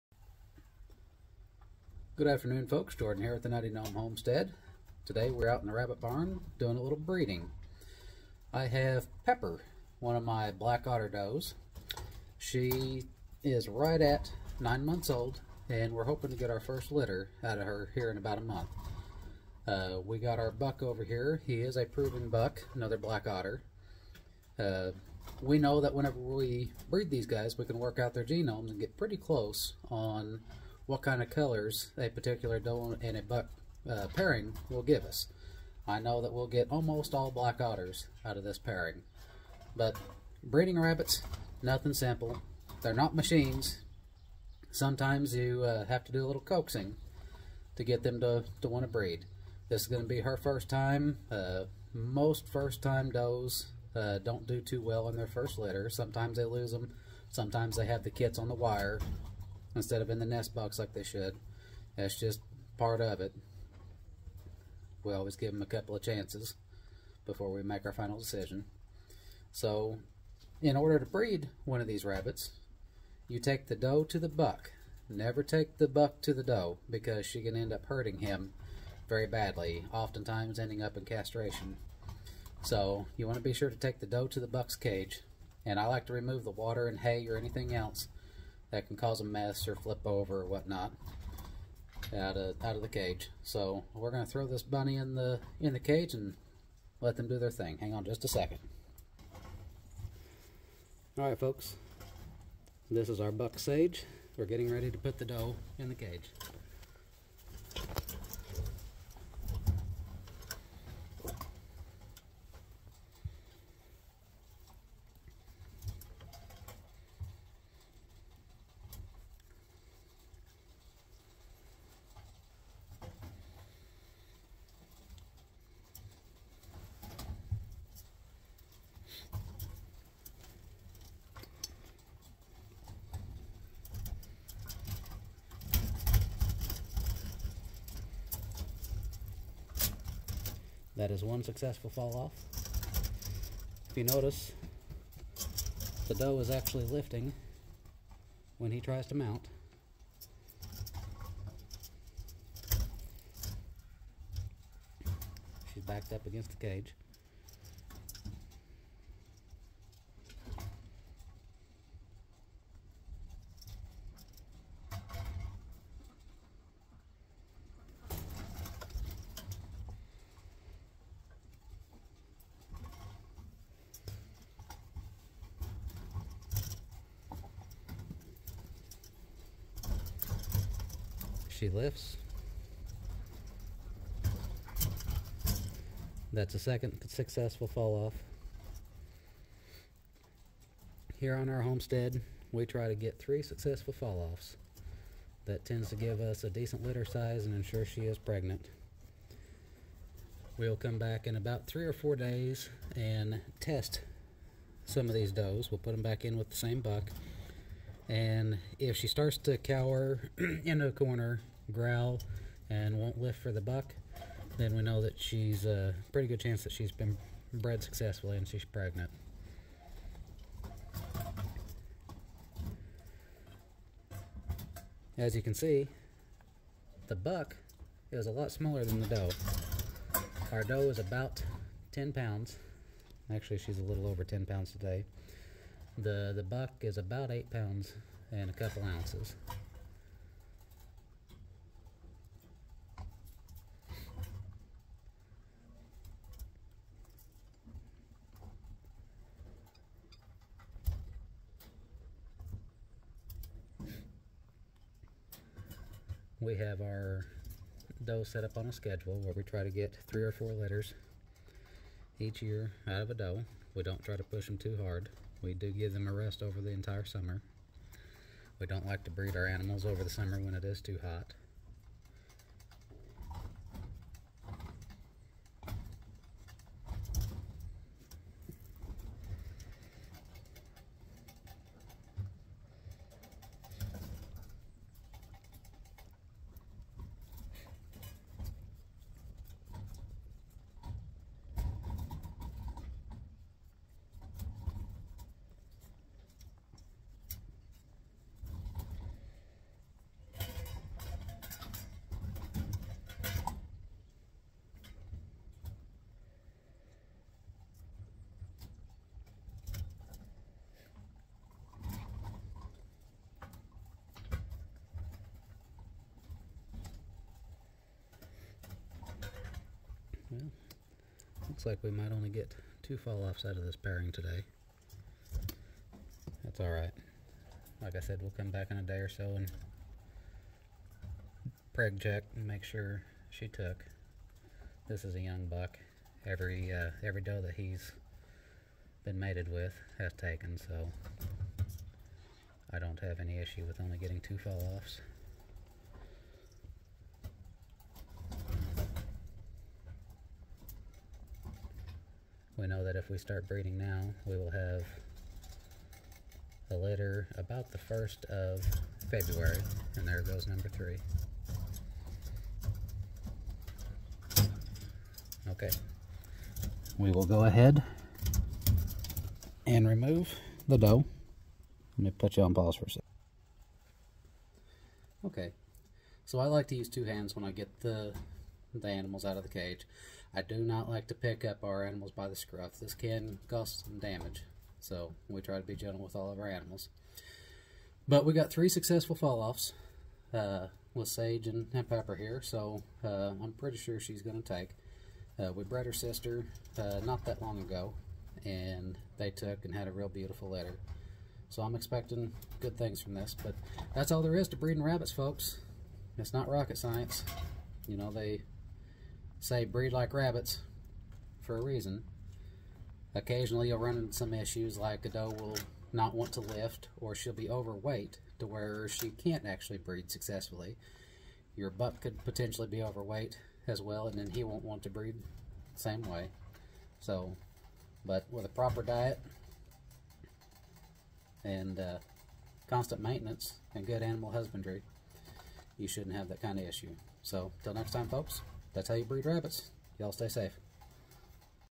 How to Breed Meat Rabbits for Beginners
Here we show the process of breeding meat rabbits. Our rabbits are of the rex variety, but these steps work for any breed of rabbit. By breeding and raising meat rabbits, we are one step closer to becoming self sufficient on our farm. We prefer knowing exactly what goes into our food and where our meat comes from.
On our homestead, we aim for 3 fall offs to increase the chances of a successful breeding. Let us know what works best for you on your homestead in the comments below!